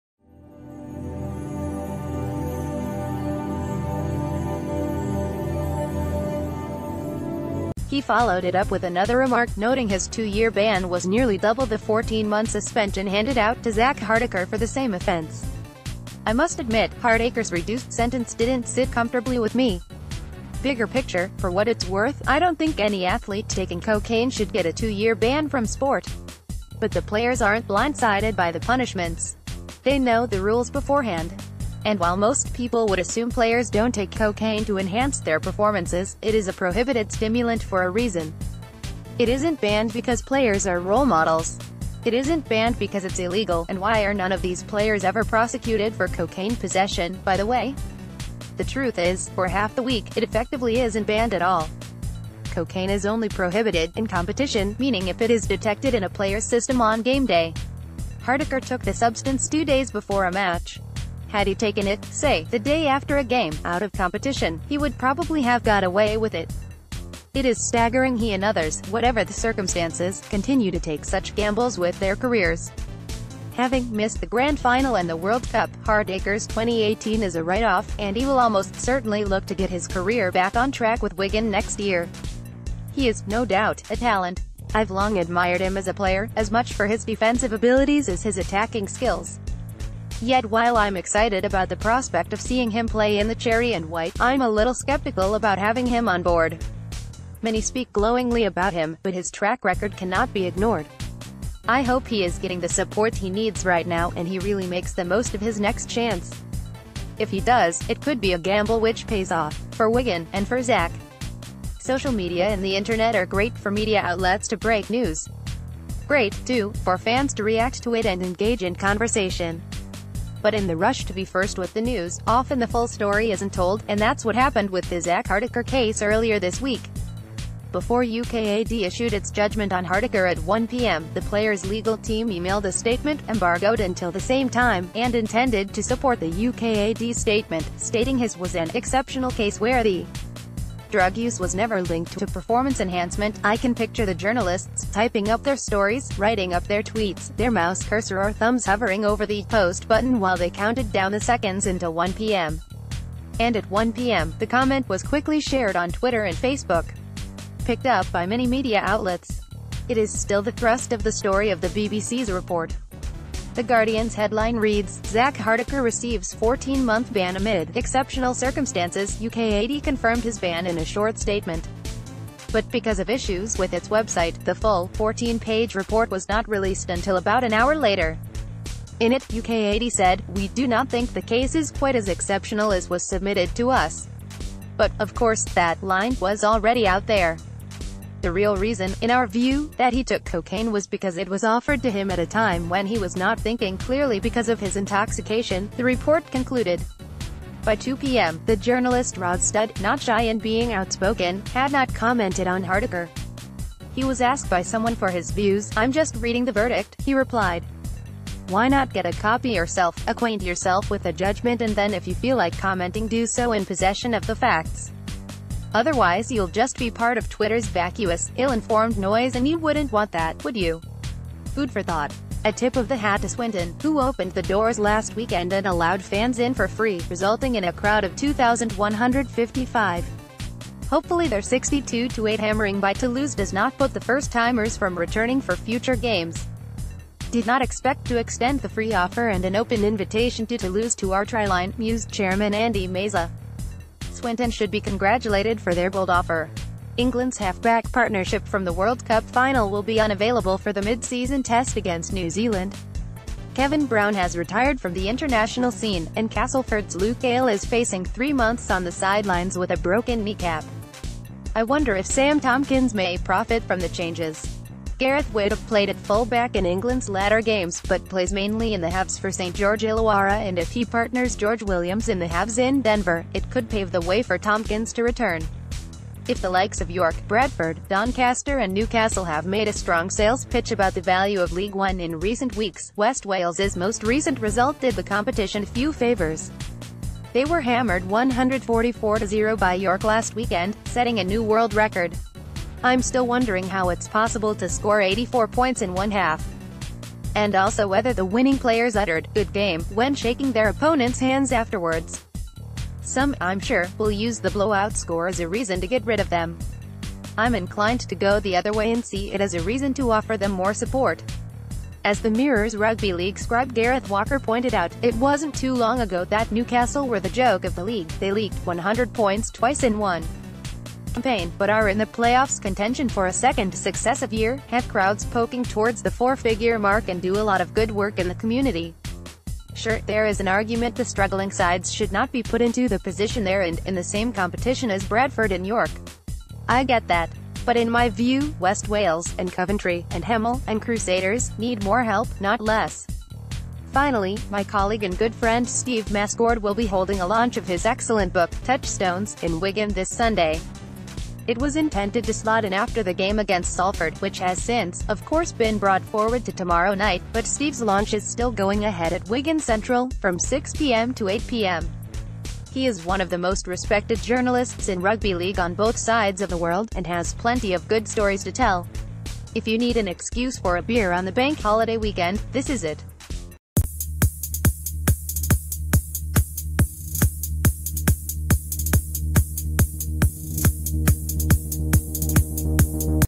Breaking News | Talking RL: ‘Zak is a talent. His signing would be a gamble’
He followed it up with another remark, noting his two-year ban was nearly double the 14-month suspension handed out to Zak Hardaker for the same offence. I must admit, Hardaker’s reduced sentence didn’t sit comfortably with me. Bigger picture, for what it’s worth, I don’t think any athlete taking cocaine should get a two-year ban from sport. But the players aren’t blindsided by the punishments. They know the rules beforehand. And while most people would assume players don’t take cocaine to enhan...